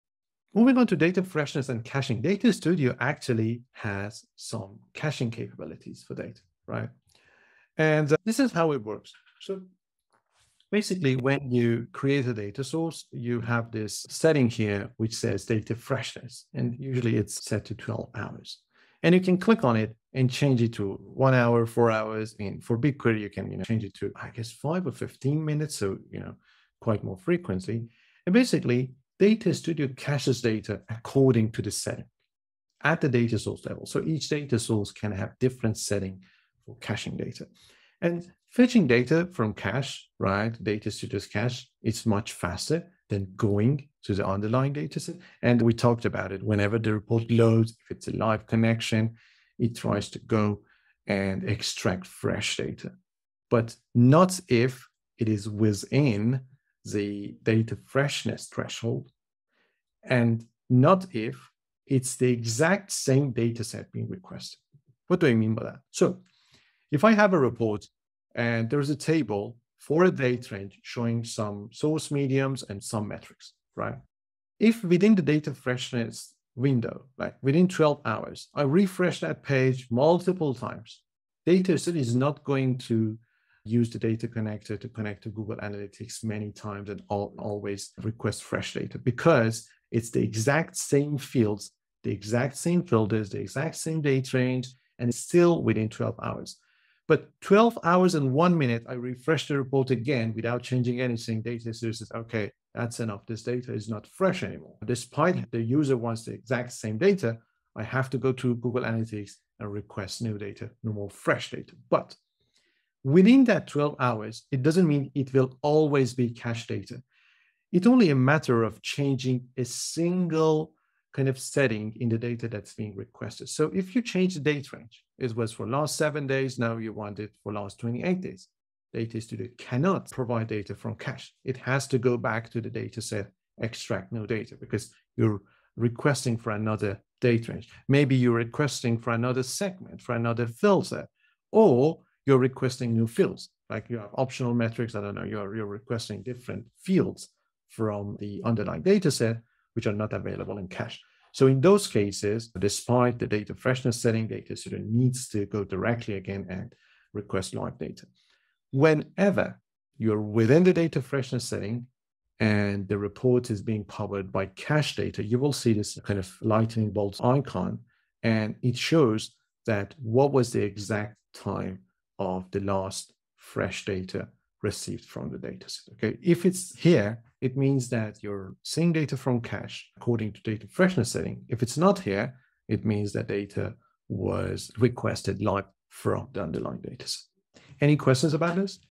3.9. Data Freshness & Caching (Looker Studio Masterclass) (Google Data Studio tutorial)
In this lesson, I'll guide you through the essentials of data freshness and caching in Looker Studio. These are critical facets to managing data and maintaining efficient report generation. Looker Studio has a set of features that let you control how fresh your data is, and how often it fetches new data from your sources.

You'll learn about changing the data freshness settings, from as short as 5 minutes up to 12 hours, and how these impact the speed of your reports. This knowledge is crucial if you need real-time reporting or want to balance between timely data and optimal system performance.

Let's explore how Looker Studio stores data temporarily, in a cache, to improve the speed of data retrieval, especially when dealing with identical datasets. This helps to avoid repeatedly connecting to your data source and requesting fresh data every time you load your report, provided you're within your set data freshness threshold.

We'll delve into the factors that can prompt Looker Studio to fetch live data despite your cache settings - like changing your date range, applying new filters, or requesting additional metrics. In short, you'll master how to manipulate Looker Studio's data freshness and caching settings to suit your reporting needs.

- Learn how to adjust data freshness settings (5 minutes to 12 hours)
- Understand the role of caching in speeding up report generation
- Explore conditions that bypass caching and fetch live data: changing date range, applying new filters, or requesting new fields
- Identify when your report is powered by cached data via a 'lightning bolt' icon
- Gain skills to balance timely data retrieval and system performance effectively

00:00 Introduction to Data Freshness and Caching
00:33 The Speed and Functionality of Data Studio's Cache
00:48 The Process of Extracting Fresh Data
01:04 The Impact of Same Data Set Requests
01:19 The Role of Data Freshness Threshold
01:52 Understanding the Usage of Cache Data
02:04 How Changing Settings Affects Data Extraction

→ Get help with Looker Studio.
→ Receive my Looker Studio tips & updates newsletter.

*Looker Studio Certification:*
When you're ready, take the final exam to test your knowledge and become a certified Looker Studio Expert →

In the data source interface, we can adjust the data freshness threshold of each data source we have for caching data and fetching data from cache.

3.9. Data Freshness & Caching
Caching in Looker Studio
Looker Studio has caching capabilities to ensure faster data retrieval. Each data source has a data freshness setting, which determines how often data is fetched from the source.

The default setting is usually 12 hours, but you can adjust it to one hour, four hours, or even five or 15 minutes for BigQuery. Each data source can have a unique caching and data fetching setting.

How Caching Works
When a report loads, Looker Studio tries to extract fresh data if it's outside the data freshness threshold. However, if the data requested is the same as previously cached data and within the data freshness window, Looker Studio will use the cached data instead.

For example, if you refresh a report multiple times within the 12-hour window without changing any settings, Looker Studio won't repeatedly request fresh data. But once the data freshness threshold is passed, it will fetch new data, even if the same data is being requested.

Note: If you change any setting in the data requested (date range, segment, filter set, or fields), Looker Studio will need to fetch new data regardless of the data freshness setting.

Identifying Cached Data
When a report is powered by cached data within the data freshness setting, you'll see a lightning bolt icon. This icon indicates the last time fresh data was received from the data source. If the icon isn't present, it means the data was requested live from the underlying data source.

Understanding data freshness and caching in Looker Studio helps you manage your data efficiently, ensuring your visualizations are both up-to-date and fast-loading.